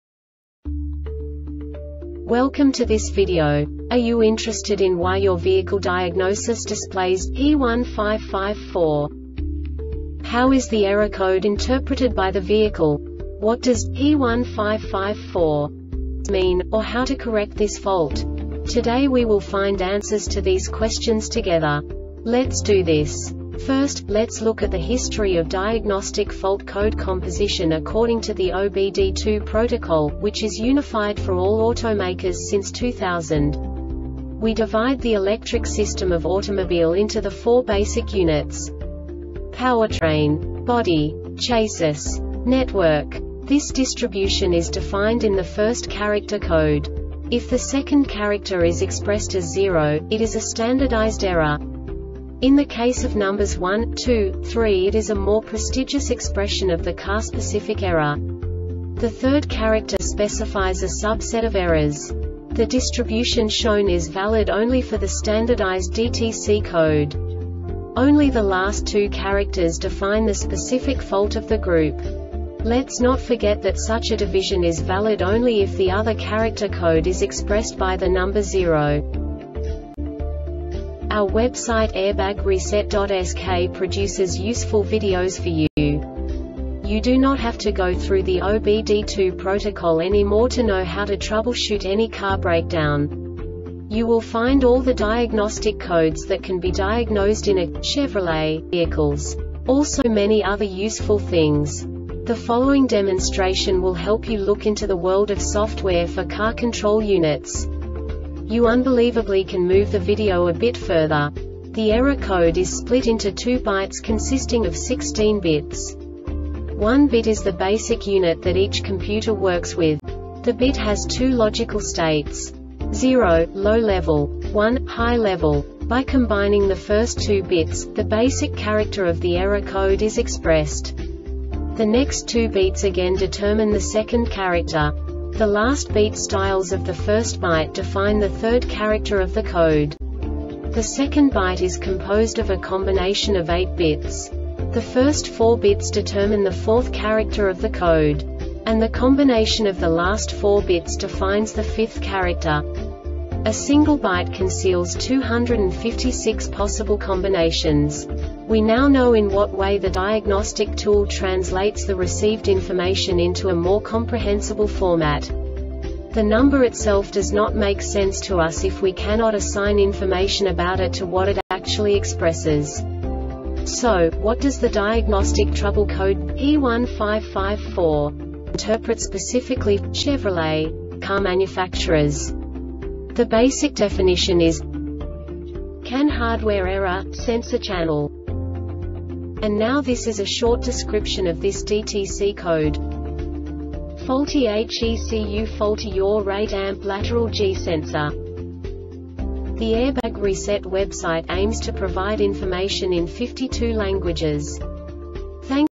DTC Chervrolet P1554 Short Explanation
Contents:
0:21 Basic DTC analysis according to OBD2 protocol standard.
1:48 Insight into programming
2:58 A diagnostically understandable description of the Diagnostic Trouble Code
3:05 Basic error definition

Make
Chevrolet
Code
P1554
Definition
CAN Hardware Error - Sensor Channel
Cause
Faulty HECUFaulty Yaw Rate  amp Lateral G sensor